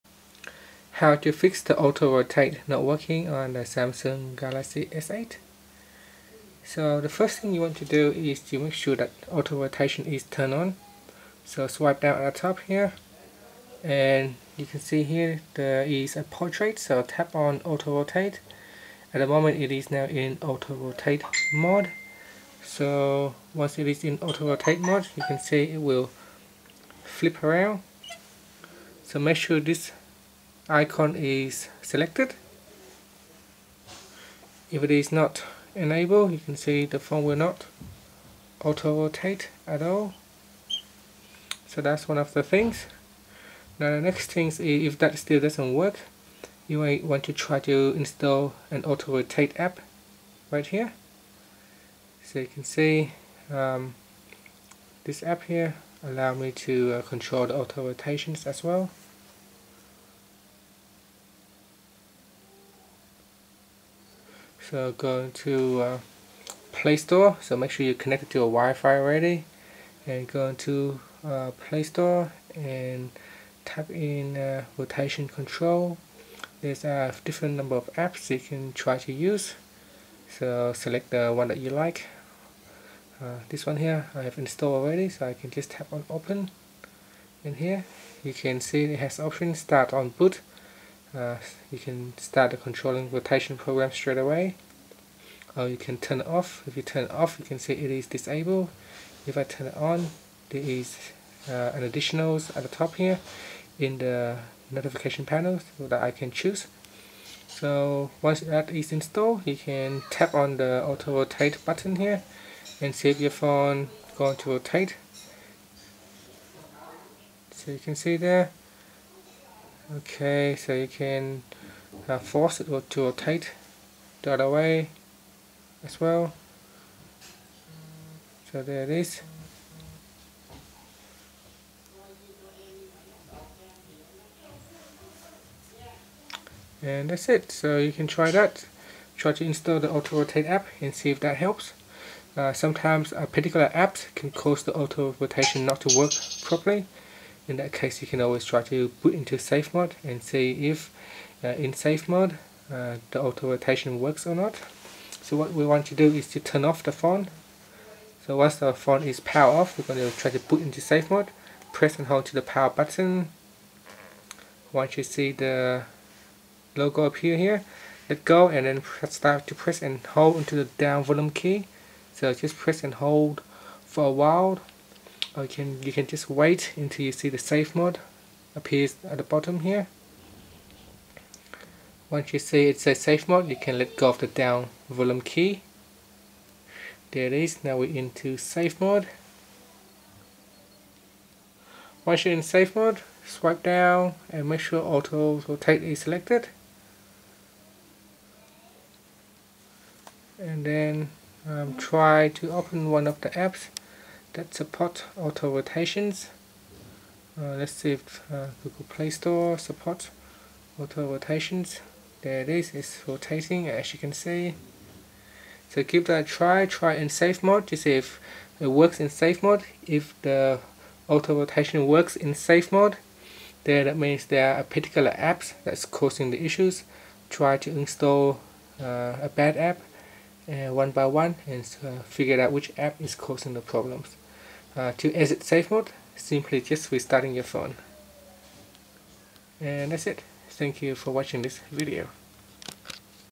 Samsung Galaxy S8: How to Fix Auto Rotate Problem
See how you can fix the auto rotate problem on the Samsung Galaxy S8.